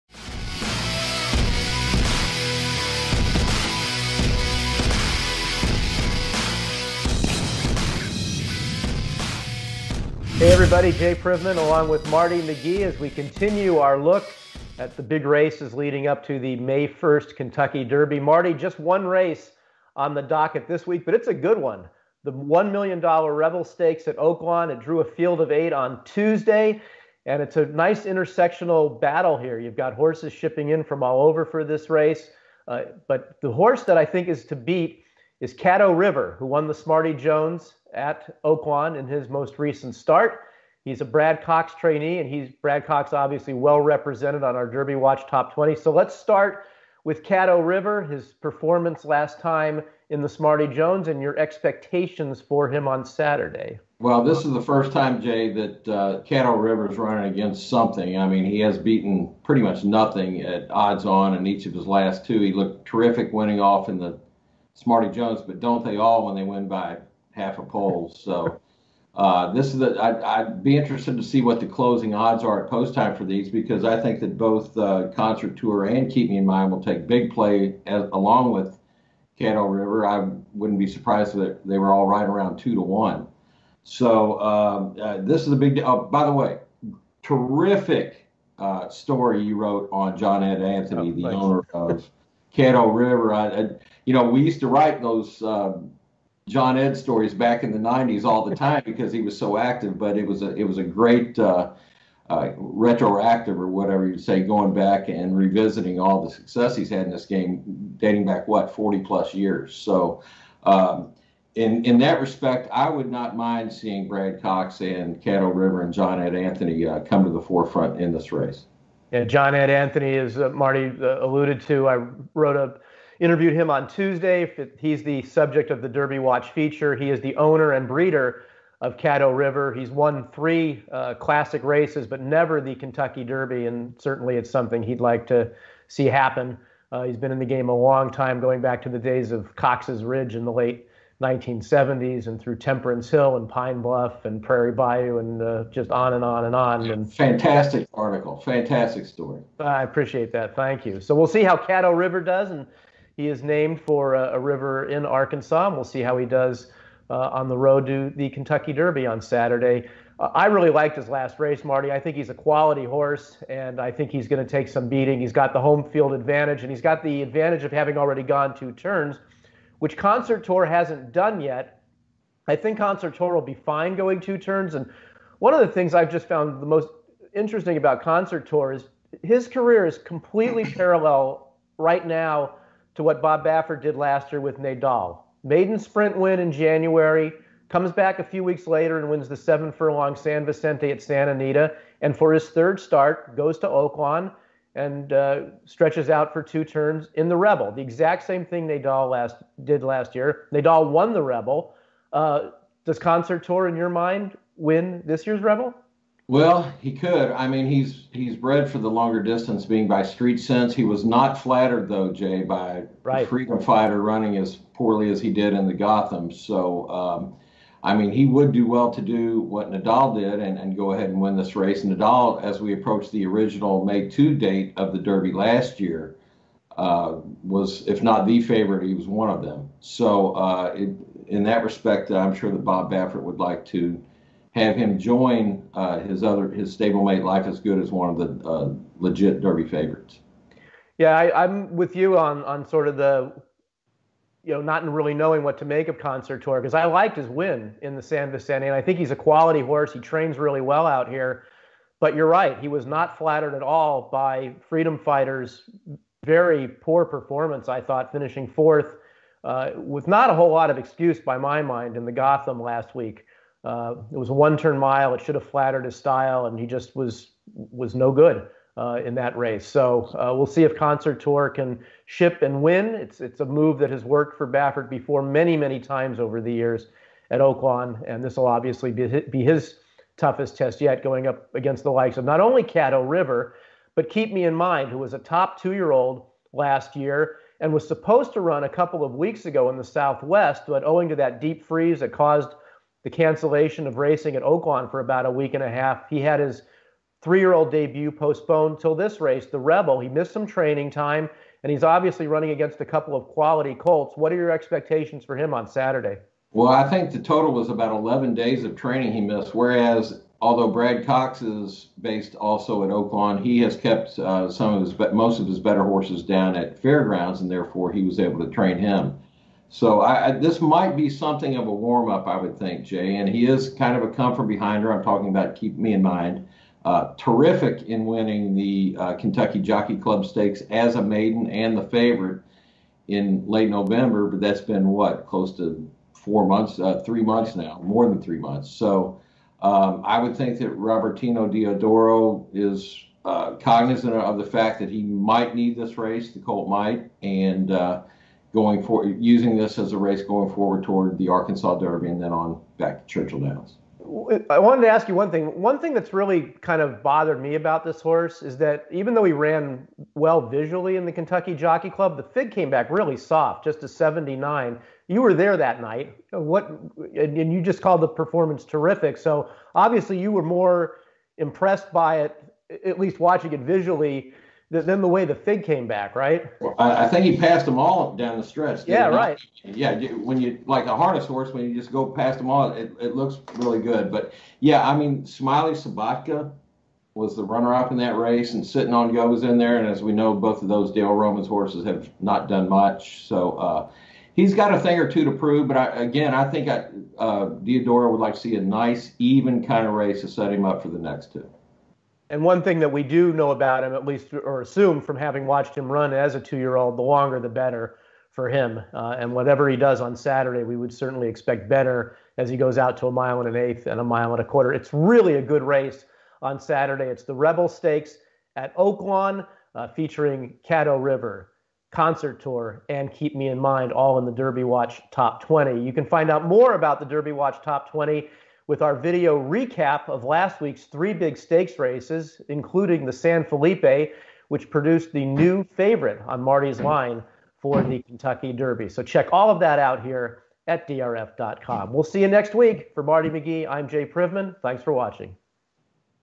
Derby Watch | March 10, 2021
On this week's edition of Derby Watch, Jay Privman and Marty McGee preview the Grade 2 Rebel Stakes at Oaklawn this Saturday on DRFTV